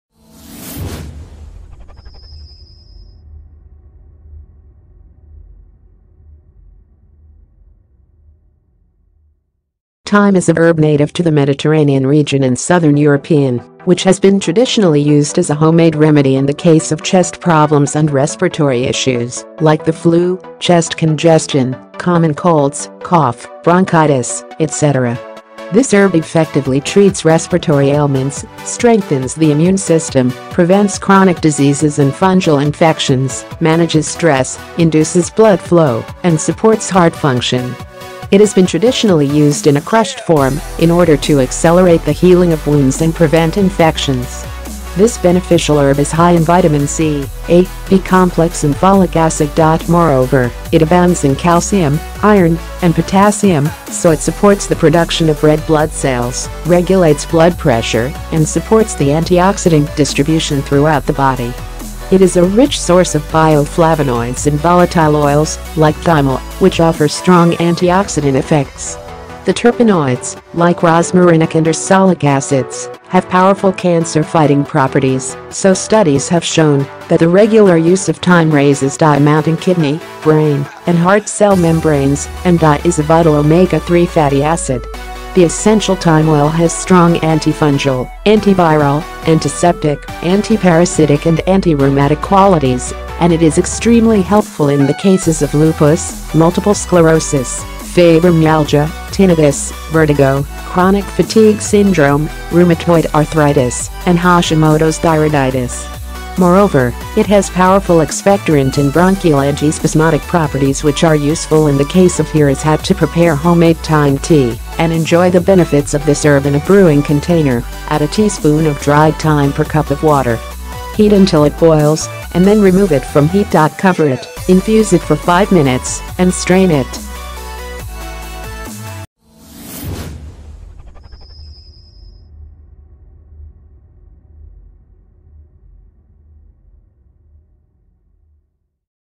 This Herbal Tea Helps in Cases of Multiple Sclerosis, Chronic Fatigue, Rheumatoid Arthritis, Fibromy
Thyme is a herb native to the Mediterranean region and southern European, which has been traditionally used as a homemade remedy in the case of chest problems and respiratory issues, like the flu, chest congestion, common colds, cough, bronchitis, etc.This herb effectively treats respiratory ailments, strengthens the immune system, prevents chronic diseases and fungal infections, manages stress, induces blood flow, and supports heart function.It has been traditionally used in a crushed form in order to accelerate the healing of wounds and prevent infections.This beneficial herb is high in Vitamin C, A, B-complex and folic acid. Moreover, it abounds in calcium, iron, and potassium, so it supports the production of red blood cells, regulates blood pressure, and supports the antioxidant distribution throughout the body.It is a rich source of bioflavonoids and volatile oils, like thymol, which offer strong antioxidant effects.The terpenoids, like rosmarinic and ursolic acids, have powerful cancer-fighting properties, so studies have shown that the regular use of thyme raises DHA amount in kidney, brain, and heart cell membranes, and DHA is a vital omega-3 fatty acid.The essential thyme oil has strong antifungal, antiviral, antiseptic, anti-parasitic and anti-rheumatic qualities, and it is extremely helpful in the cases of lupus, multiple sclerosis, fibromyalgia, tinnitus, vertigo, chronic fatigue syndrome, rheumatoid arthritis, and Hashimoto’s thyroiditis.Moreover, it has powerful expectorant and bronchial antispasmodic properties which are useful in the case of:Here is how to prepare homemade thyme tea and enjoy the benefits of this herb:In a brewing container, add a teaspoon of dried thyme per cup of water.Heat until it boils, and then remove it from heat. Cover it, infuse it for 5 minutes, and strain it.